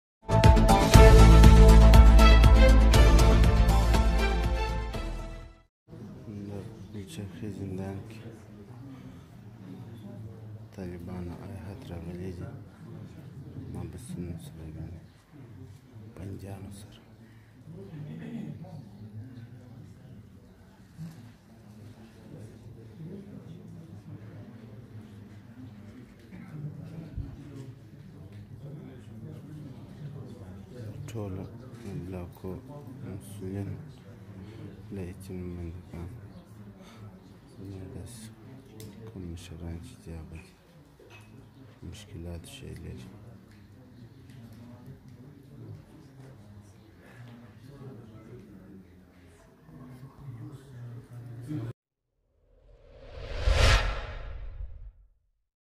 طالبانو دمولوي ضیاءالدین په مشري د پلچرخي زندان ته خپل یو پلاوی لېږلی ترڅو د خپلو بندیانو څخه له نژد
طالبانو دمولوي ضیاءالدین په مشري د پلچرخي زندان ته خپل یو پلاوی لېږلی ترڅو د خپلو بندیانو څخه له نژدې لیدنه وکړي او له ستونځو یې مخامخ ځان خبر کړي دا لمړۍ ځل دی چي دطالبانو پلاوی له خپلو بندیانو سره مخامخ ویني.